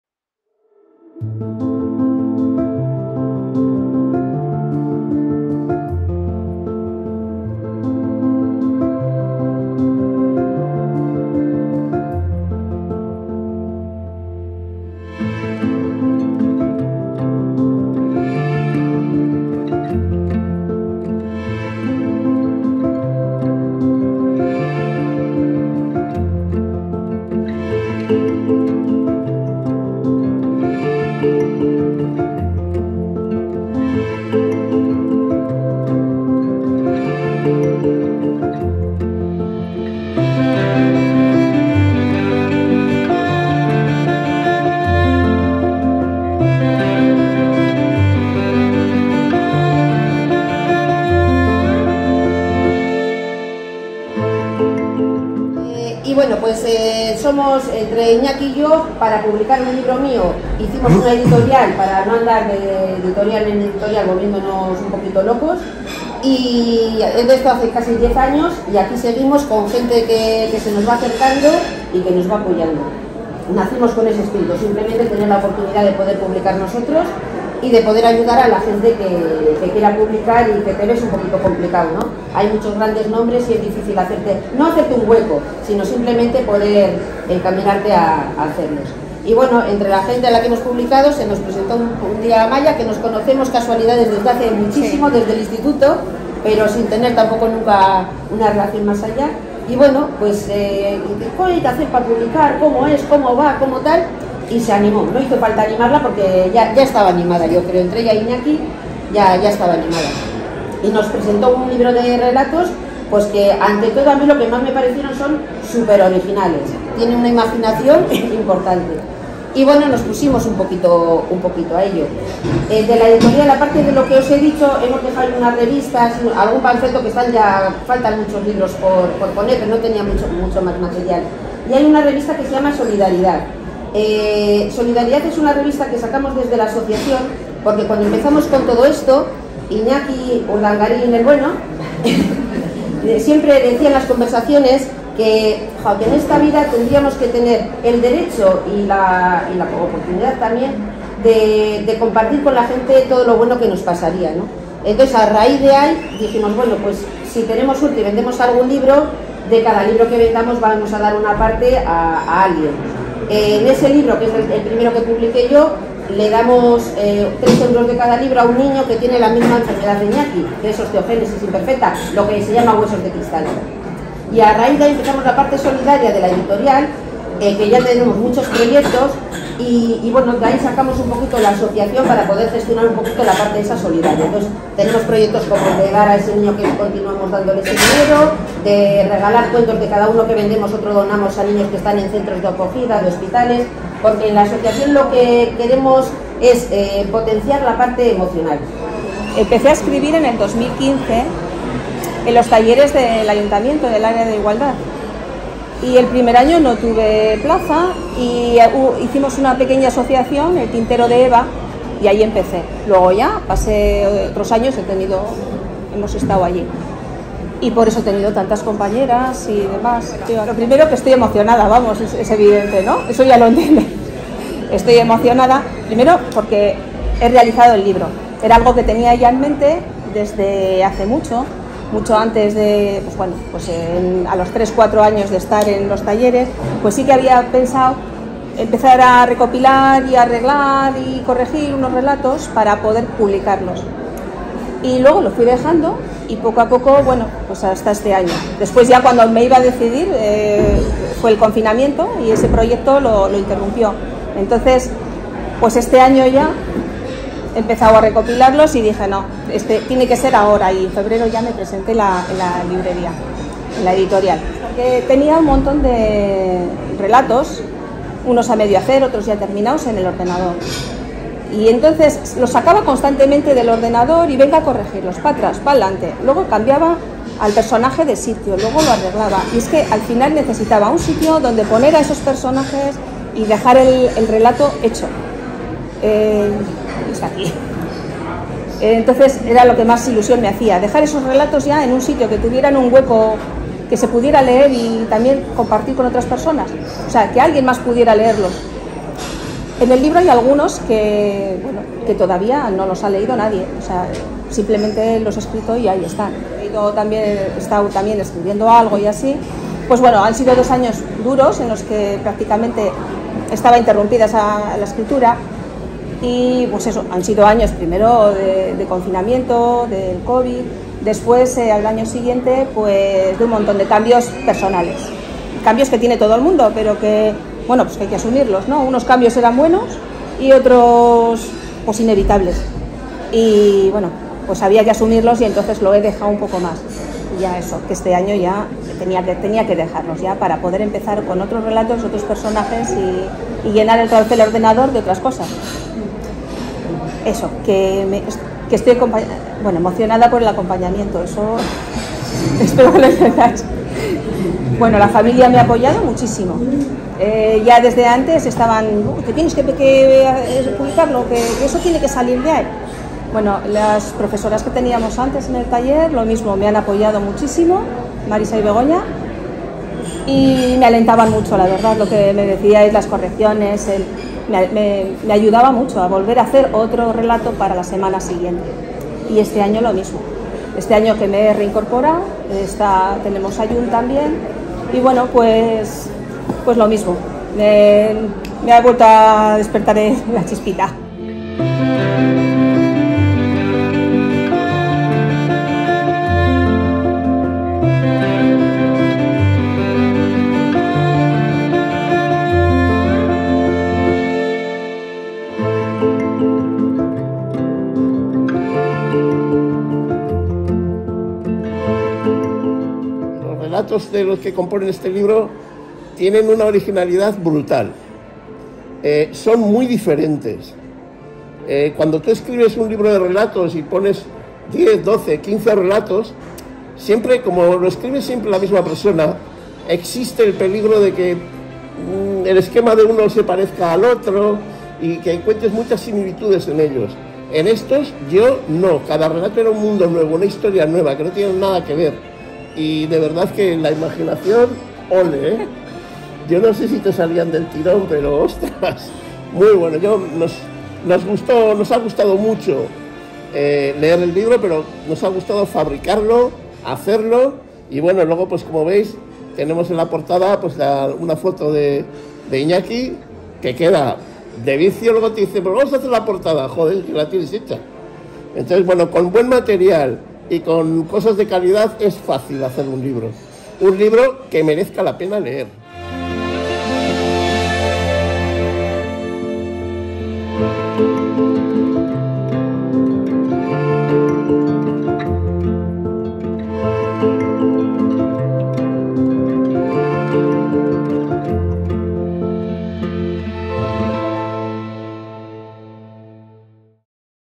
PRESENTACIÓN OJOS DE MARIPOSA
E S P E C T A C U L A R la presentación de OJOS DE MARIPOSA en el Café IRUÑA de Bilbao! Sala abarrotada, público entregado, Amaia Martínez Aldecoa, autora del libro, emocionada y valiente y yo encantado de poder formar parte de momentos tan especiales que hacen realidad sueños soñados en atardeceres llenos de esperanzas. ¡Una pasada!!! ¡Enhorabuena
Amaia!!! lo hiciste fenomenal!!! ¡Gracias al Café IRUÑA por TODO!!! y a los que compartisteis con nosotros este evento... gracias!!! Veis lo poderosos que sois? A disfrutar y seguir... haciendo camino!